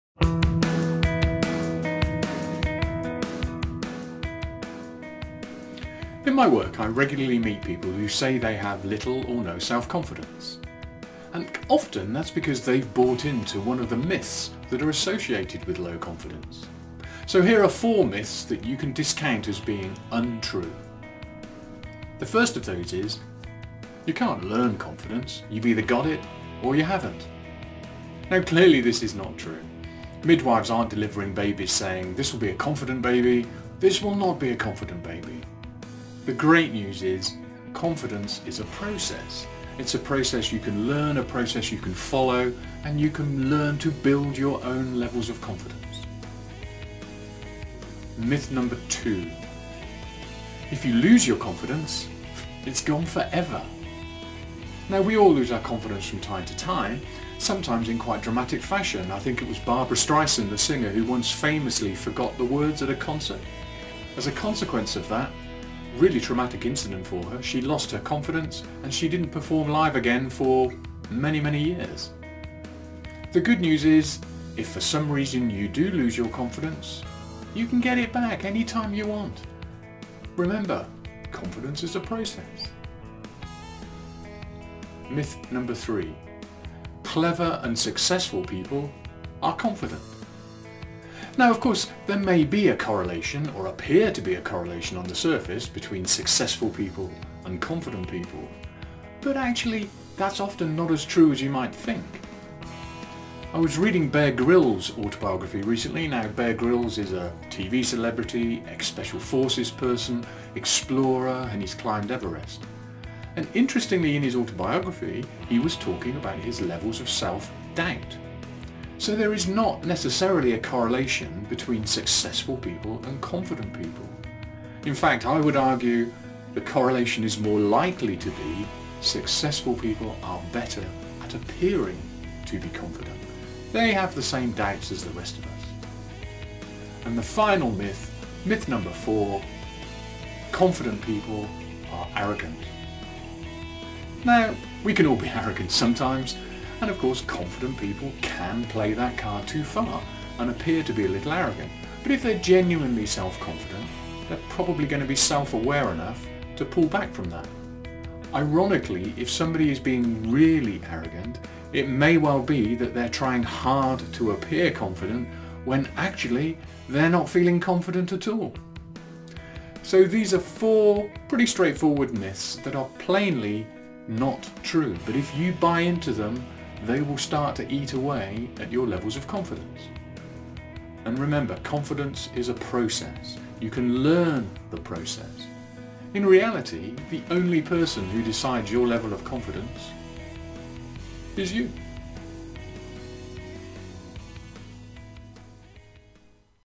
Self Confidence Myth Buster
Low Self confidence levels is an issue for many people. Often they have inadvertently bought into one of the common myths that surround self-confidence, and as a result they are not achieving what they want to in life. Here are four of the most common confidence myths that, if you decide to accept them, will eat away at your confidence levels, lower your performance and stop you succeeding. They form the basis of limiting beliefs that hold us back.

The good news - self confidence is a process. You can learn to grow your confidence.